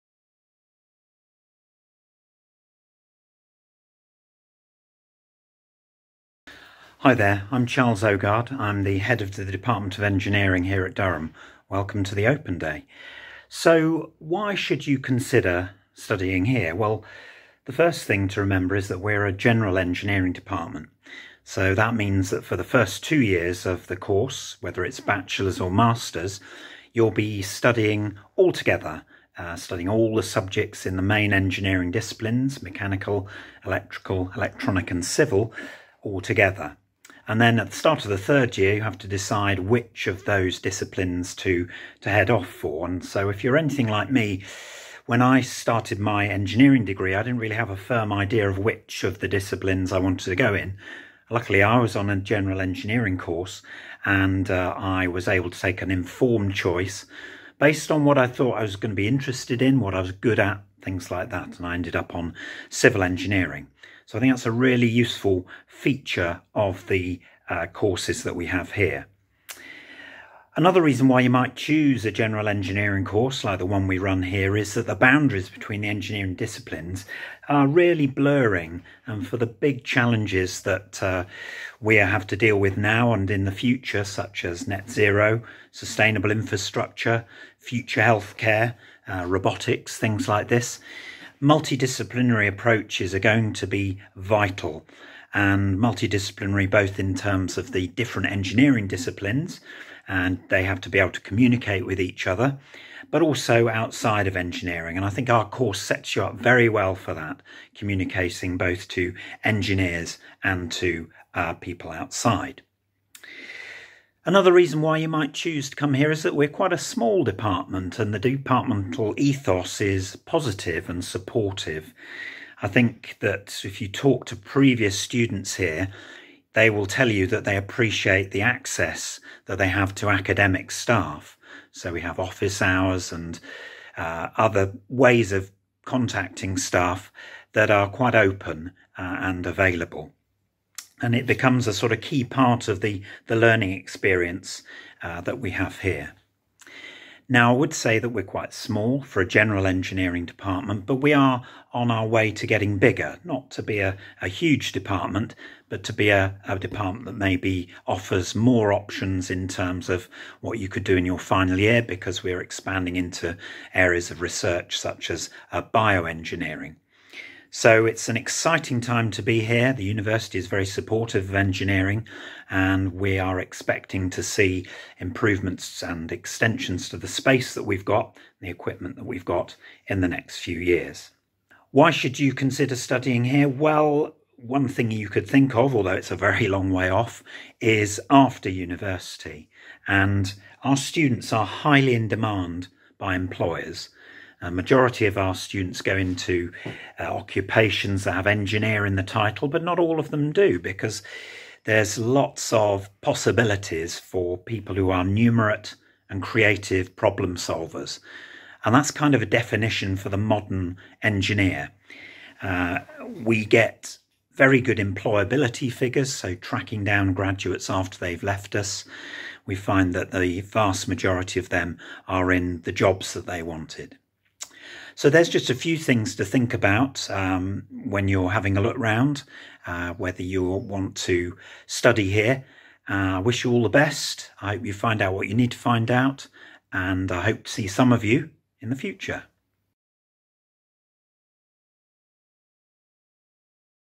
Welcome to the department of engineering | Open Day Video
The Department of Engineering operates under a "General Engineering" style for the first two years of the degree. In the third year, our undergraduate students specialise in one of several disciplines (Civil, Mechanical, Electrical, Electronics). Students doing an integrated Masters course can specialise further in their fourth year.

Are you interested in studying Engineering? Why not come along to an open day or have a look at our undergraduate webpage: